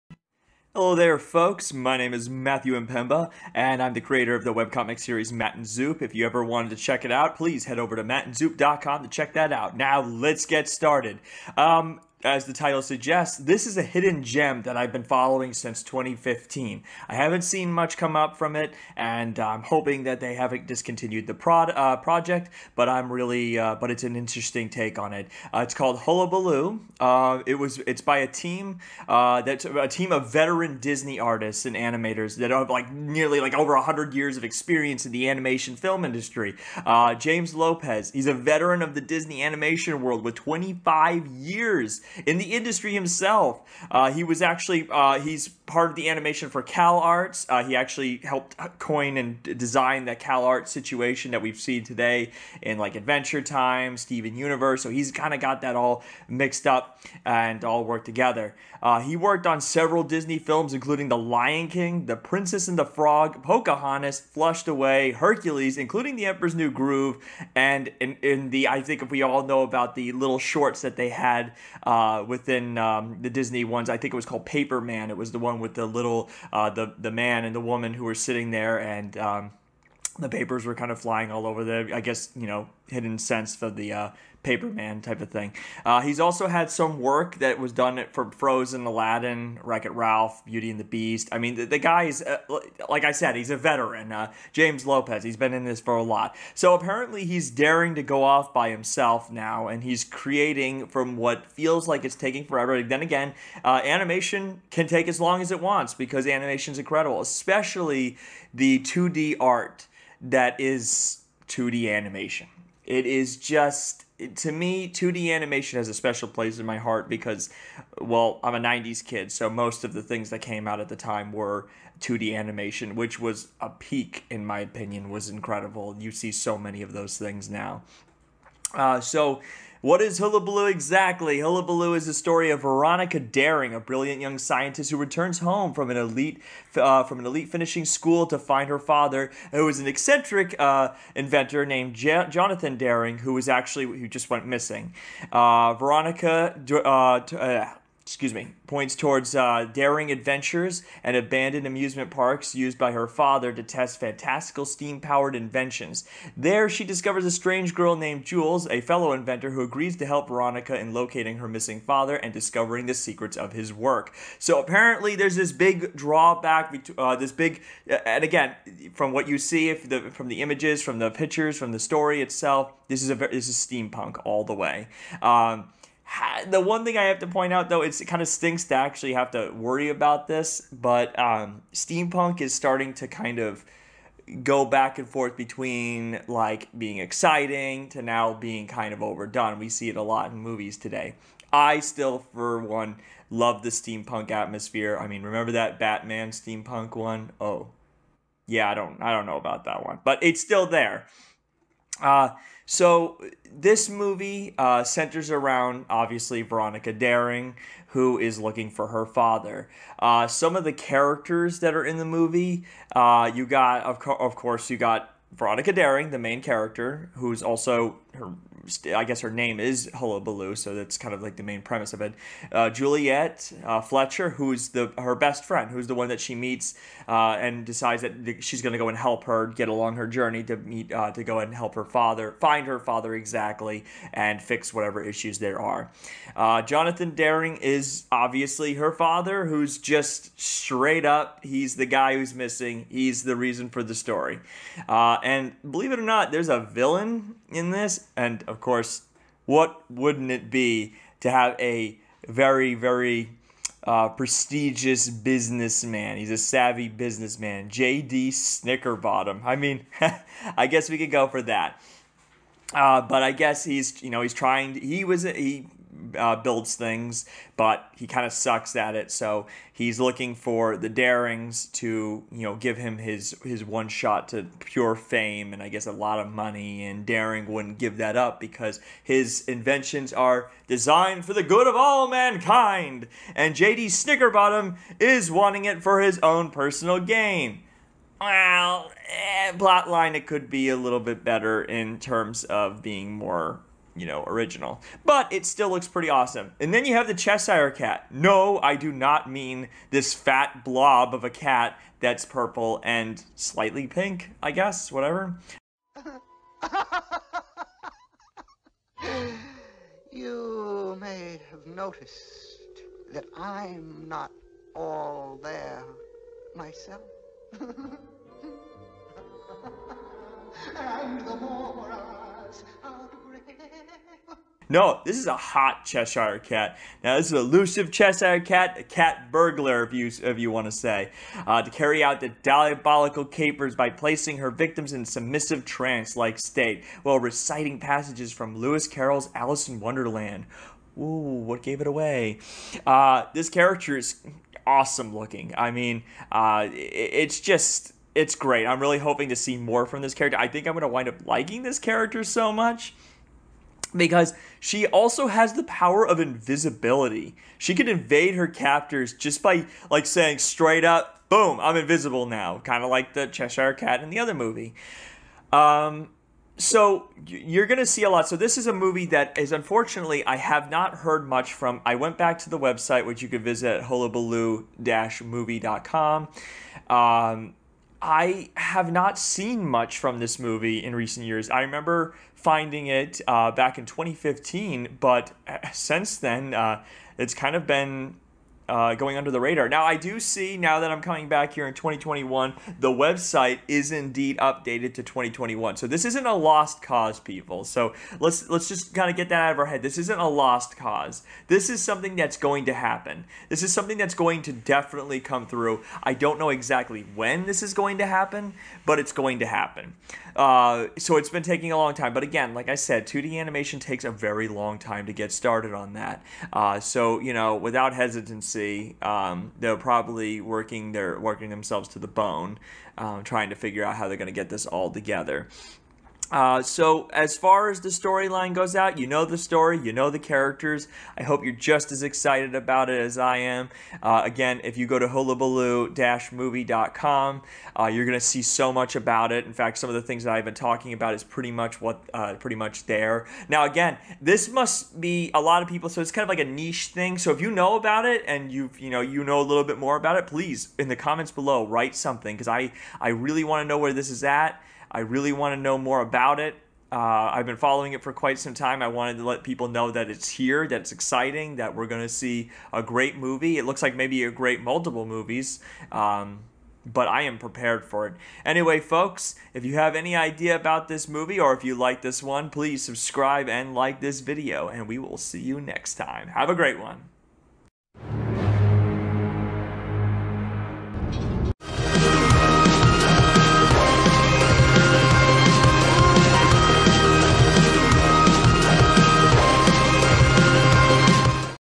Disney has another hidden gem!?
If you haven't heard about it you probably will, a new 2-D animated movie called Hullabaloo which has been in the works for quite some time and is crowd funded.

James Lopez a veteran is leading a group of highly talented animators to bring this fantastical story to life that is nearly 7 years in the making! or follow me for more excitement from the animation industry!

Story Synopsis:

Hullabaloo is the story of Veronica Daring, a brilliant young scientist who returns home from an elite finishing school to find her father--the eccentric inventor Jonathan Daring--missing without a trace! The only clue left behind points Veronica toward Daring Adventures, an abandoned amusement park used by her father to test his fantastical steam-powered inventions.

There she discovers a strange girl named Jules, a fellow inventor who agrees to help Veronica in locating her missing father and discovering the secrets of his work.Together, Veronica and Jules learn that Jonathan Daring has been kidnapped by a mysterious group of influential persons, who seek to use his latest invention for nefarious purposes. These villains are wealthy and influential and neither Veronica nor Jules can stop them openly. But determined to save her father and holding true to the family creed that technology should be used for the good of all, not the greed of some, Veronica assumes the secret identity of "Hullabaloo", a goggled crusader who uses wits and science to combat evil and oppose the nefarious conspiracy that has taken her father.But the question remains: what are the villains up to?

 What do they plan to do with Jonathan Daring's mysterious new technology? And will Veronica and Jules be able to stop them in time?